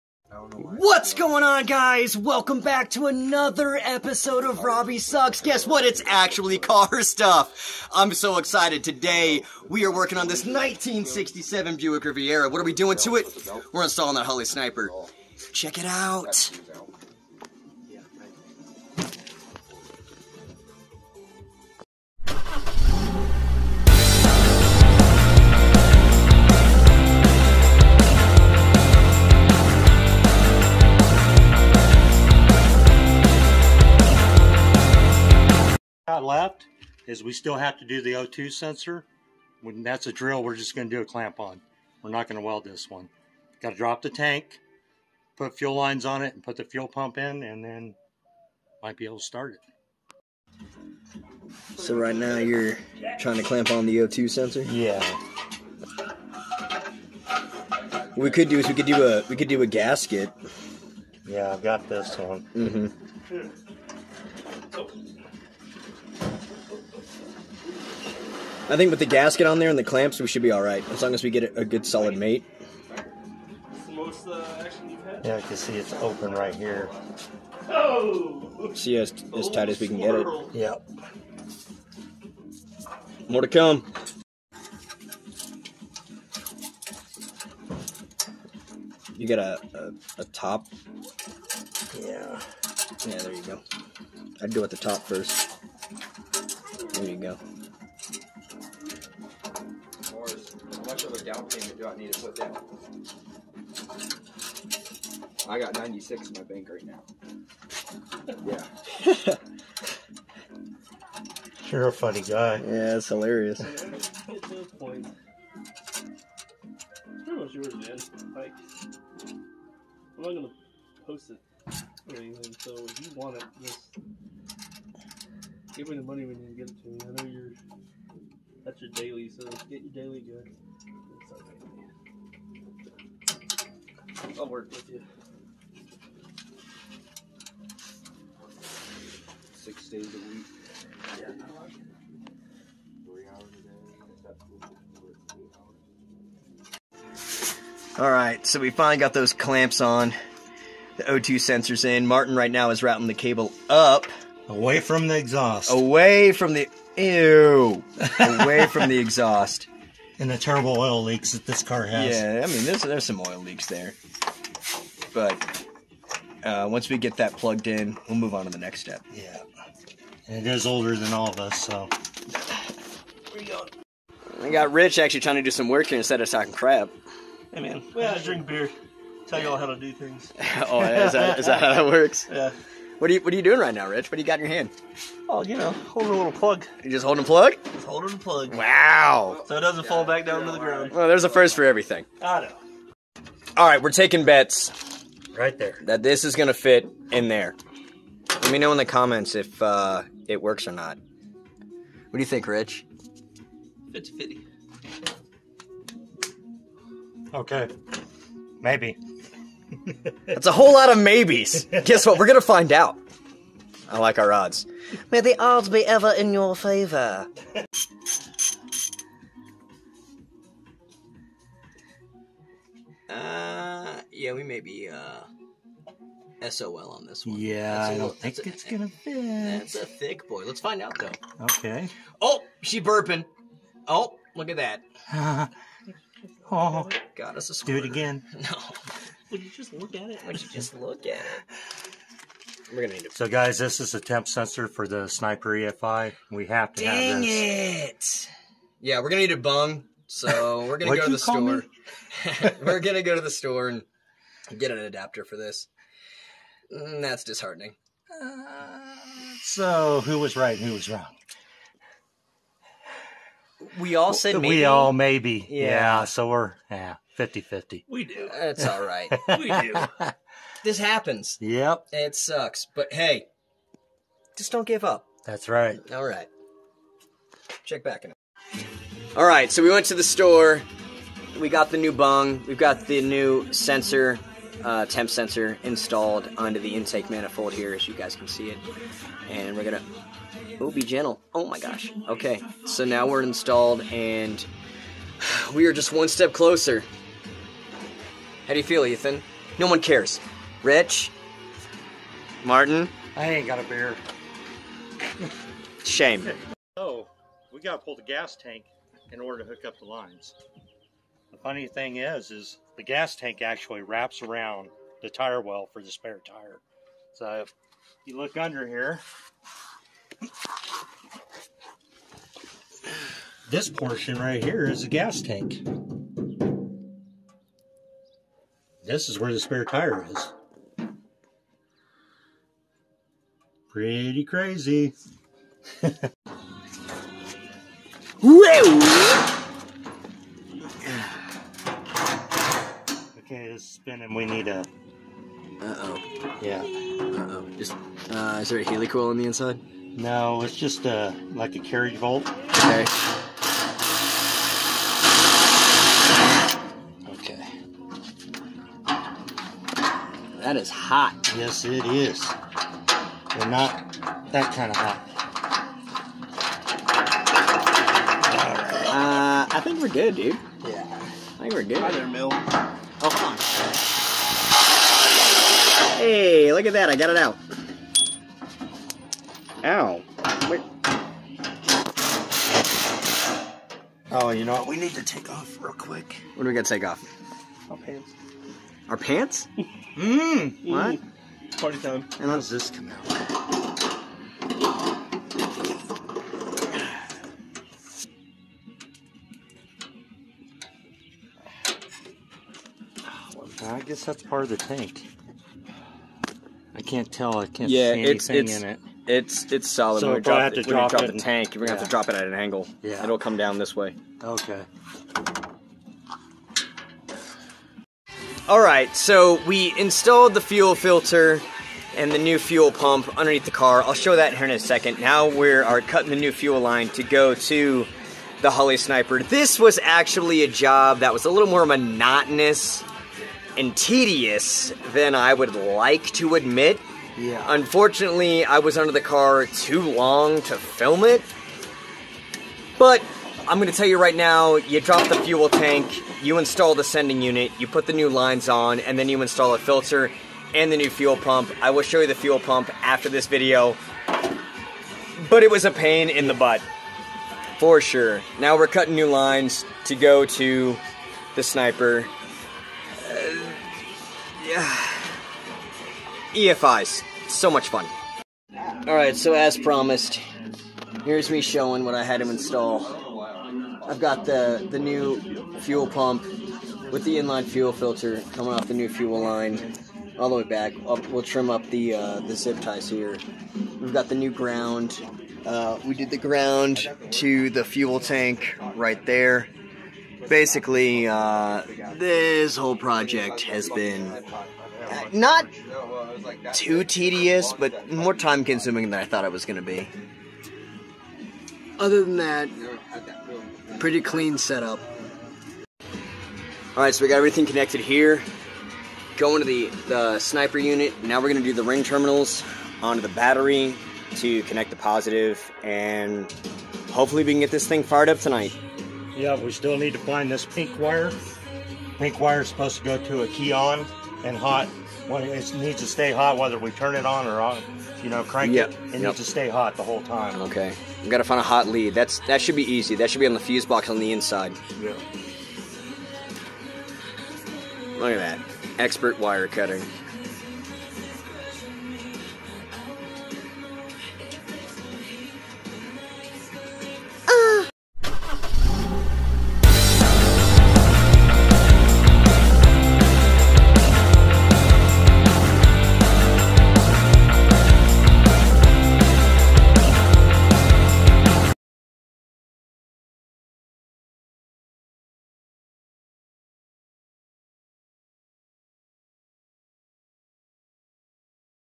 Boating down the Riviera!
putting a a Holly Sniper EFI on a 1967 Buick Riviera holly EFI restomod #1967